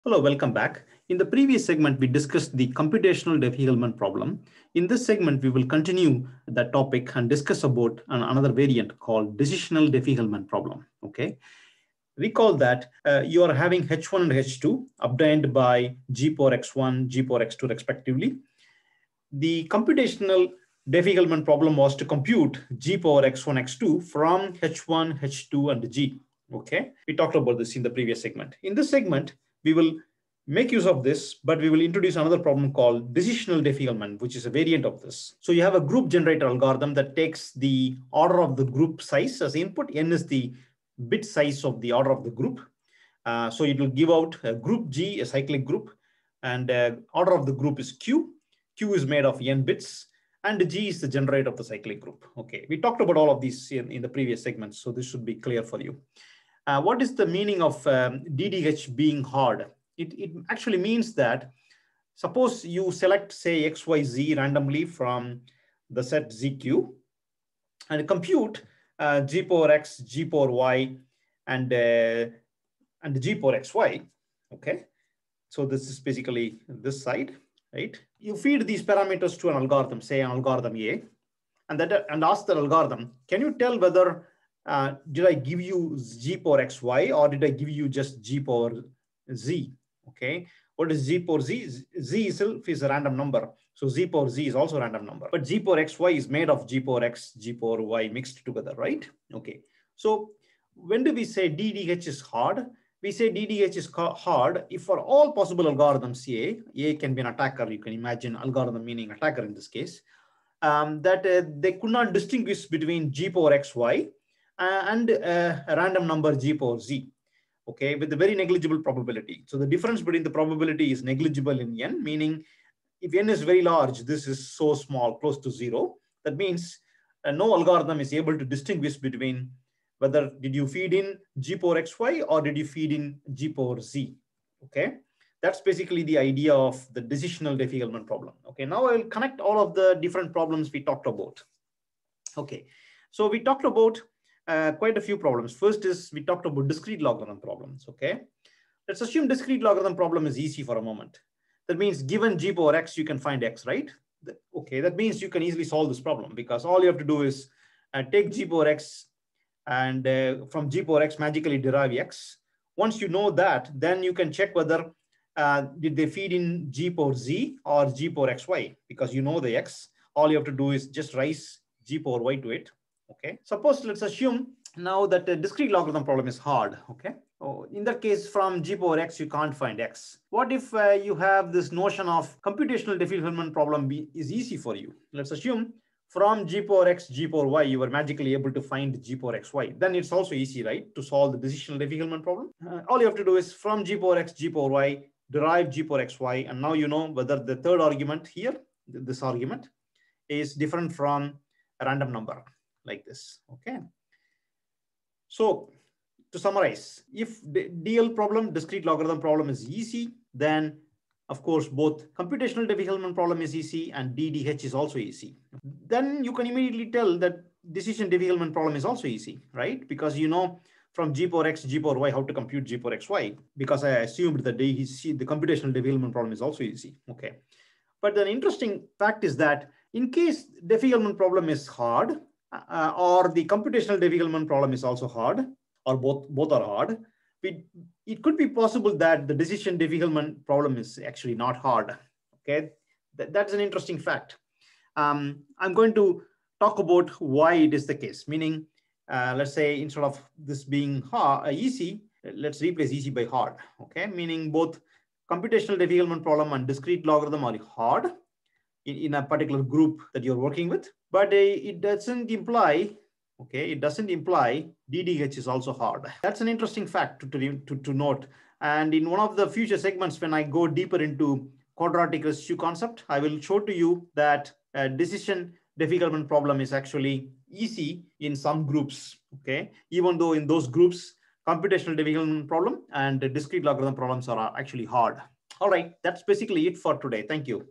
The Decisional Diffie-Hellman (DDH) problem
We formalize the DDH notion.